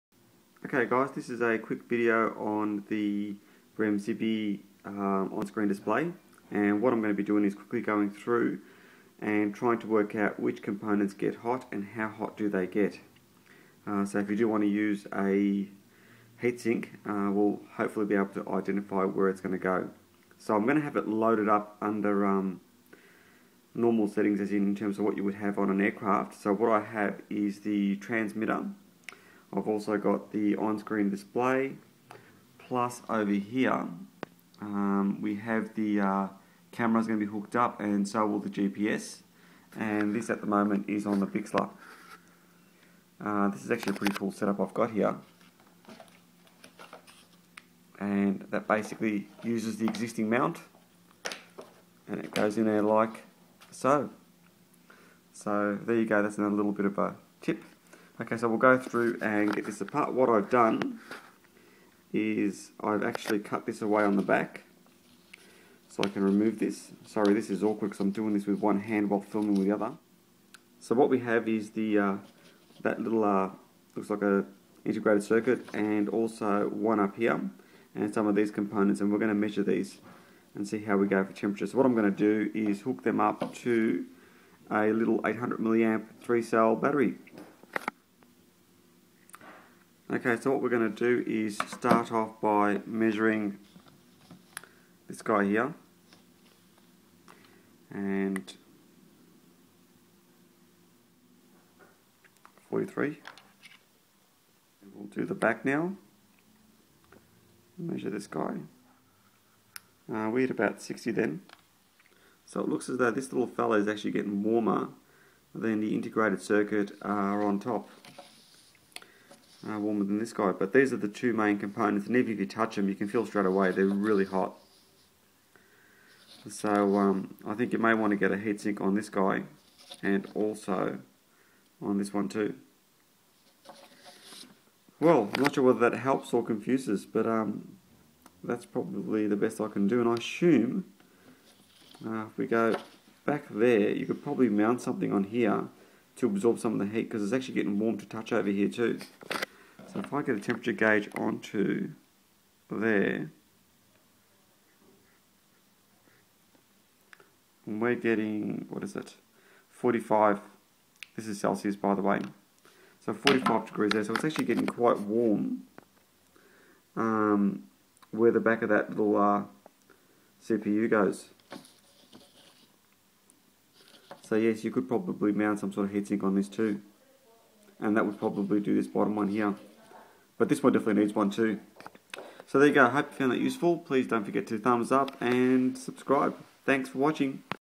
Remzibi's OSD FPV Copy - Cooling Components - On Screen Display GPS System
Looking at which parts are warming up and identifying which need too be cooled